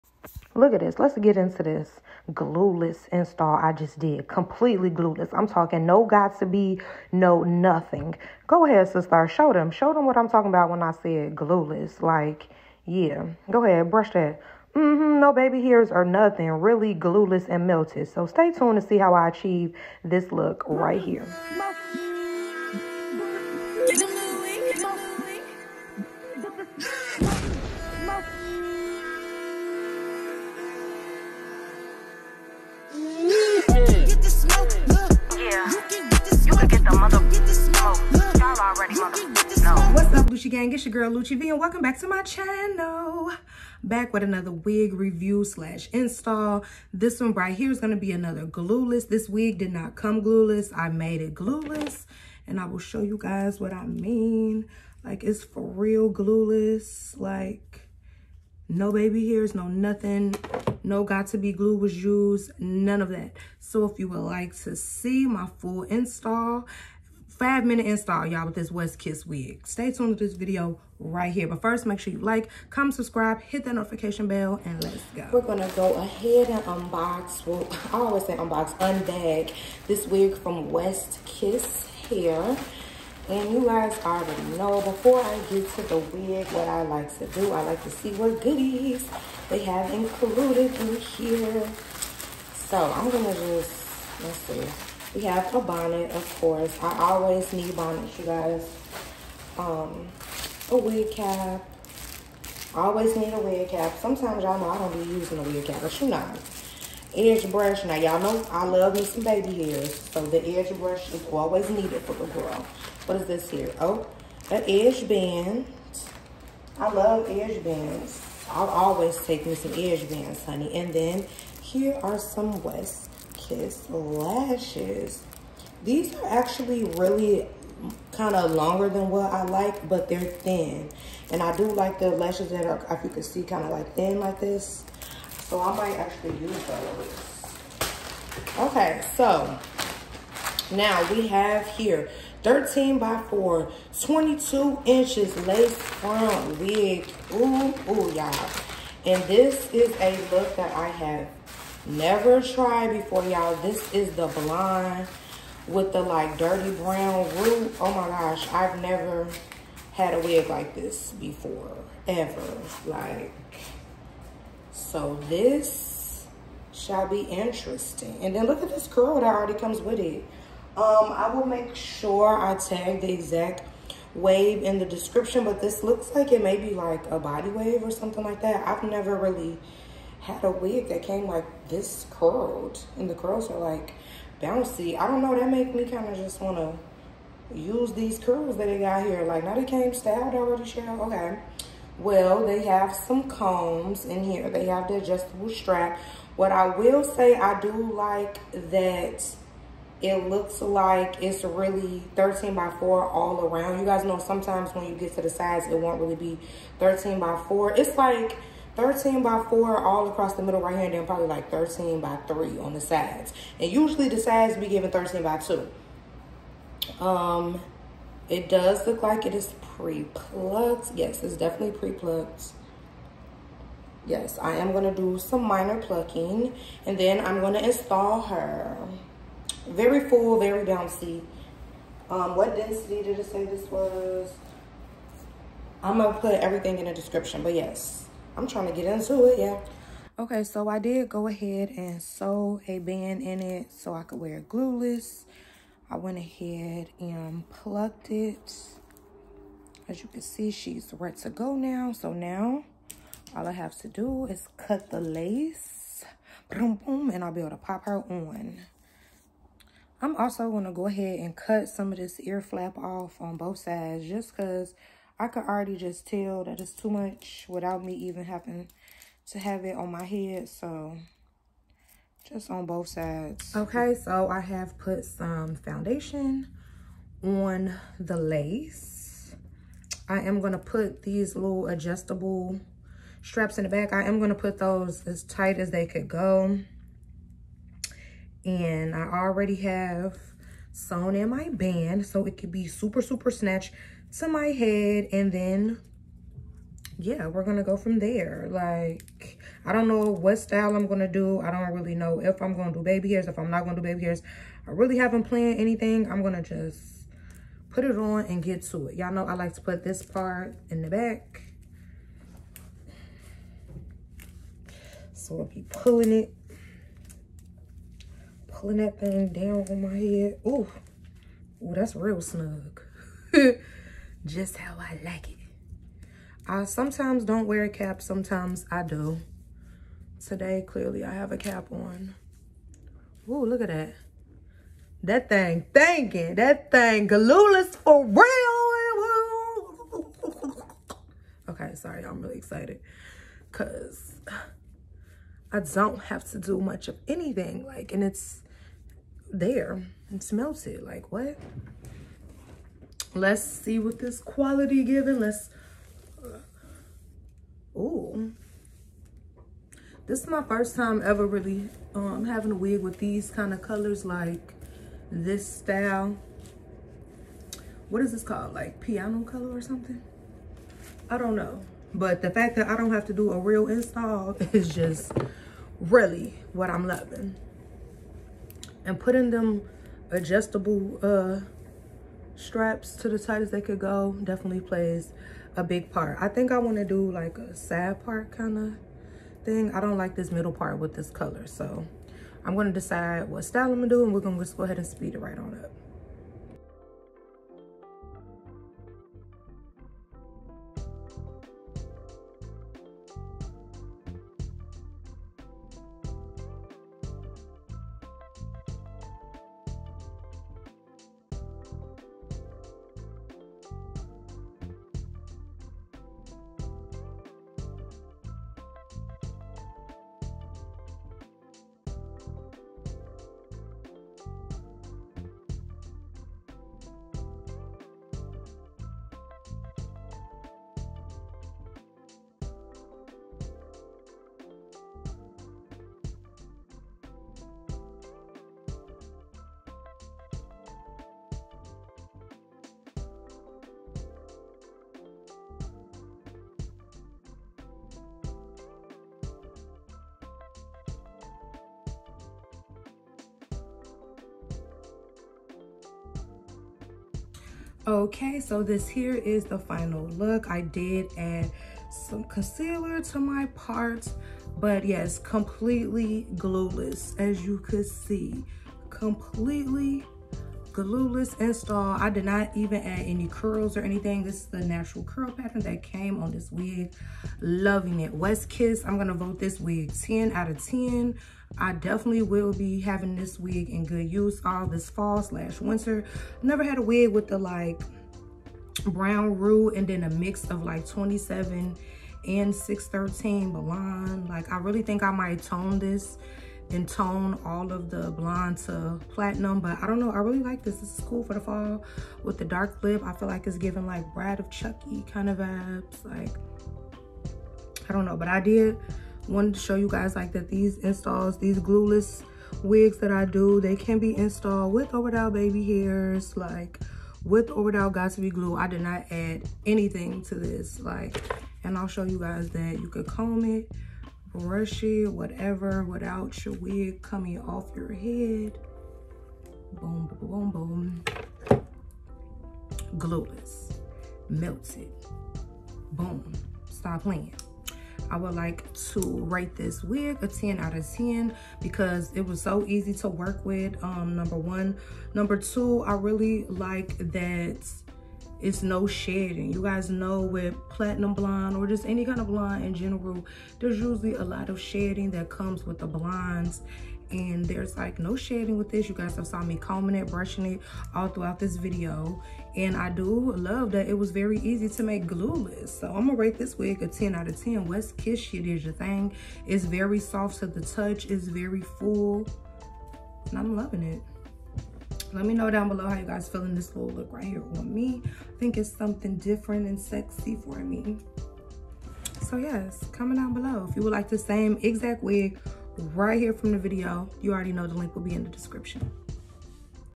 5 MINUTE WIG INSTALL FEAT. WESTKISS HAIR
Hair details: 22inches P27/613 highlight with brown roots frontal wig 180% density

Stream my album!